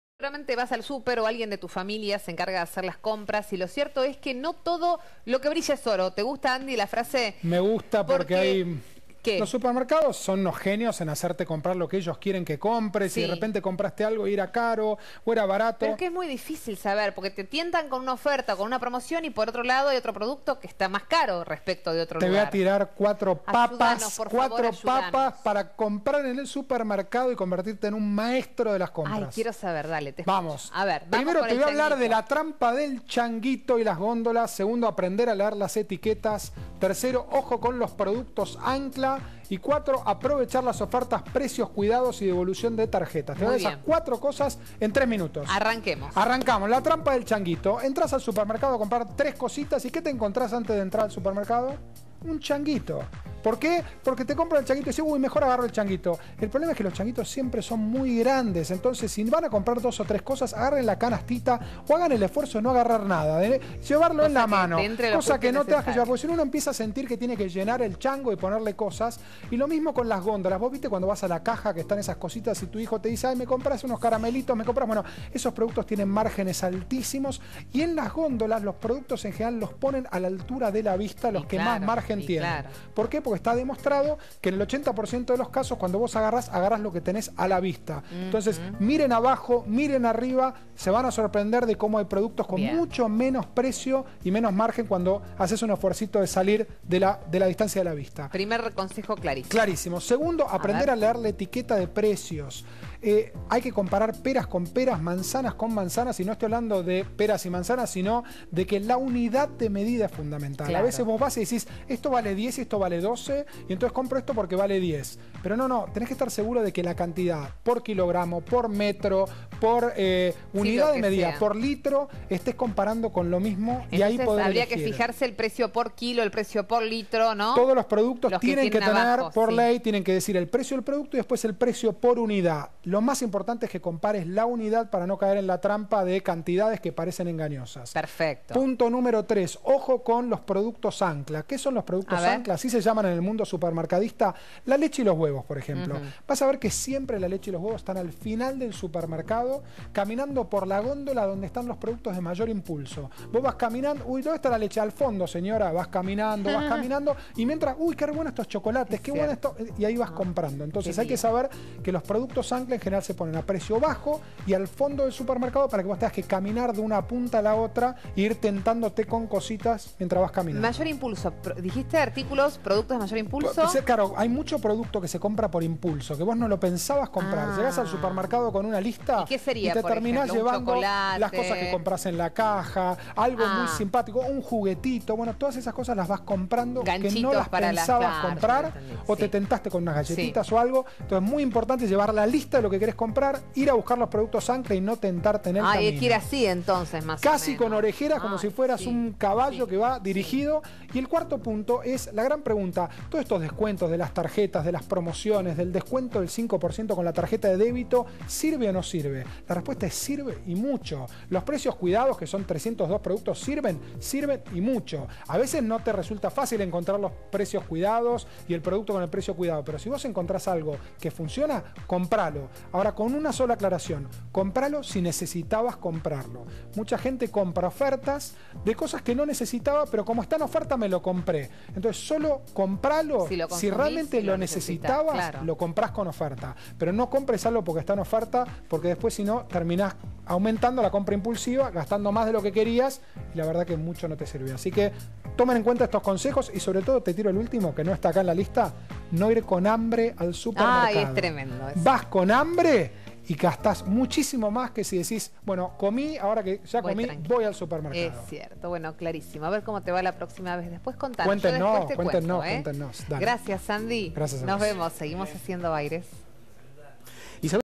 Andy Freire - Claves para comprar mejor en el súper
Andy Freire es emprendedor del ámbito empresarial y social. Cofundó Quasar Ventures, OfficeNet, Axialent y Restorando, y presidió la Fundación Endeavor en Argentina. Además es columnista en Telefé y en Radio Metro 95.1.